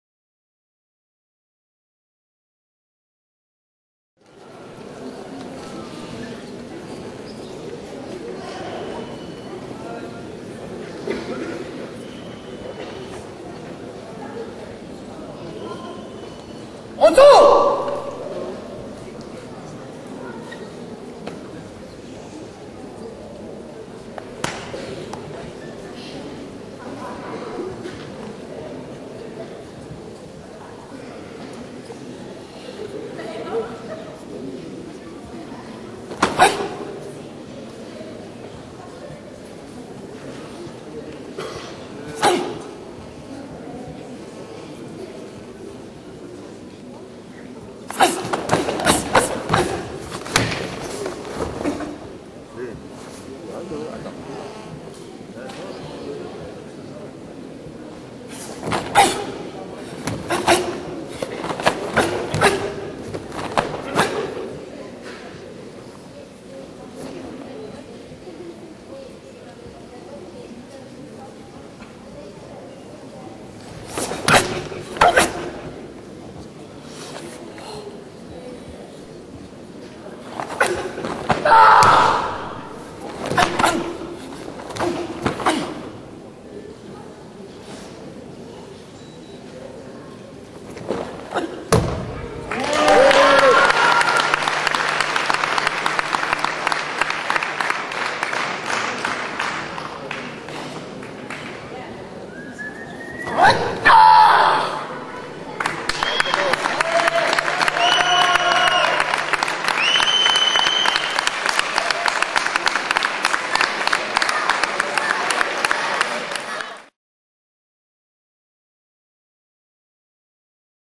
unsu
Чемпионат Бельгии 2003 Лефевр ката унцу. Видео публикуется с разрешения самого Джуниора Лефевра, для популяризации его семинара в Москве, информация о семинаре здесь: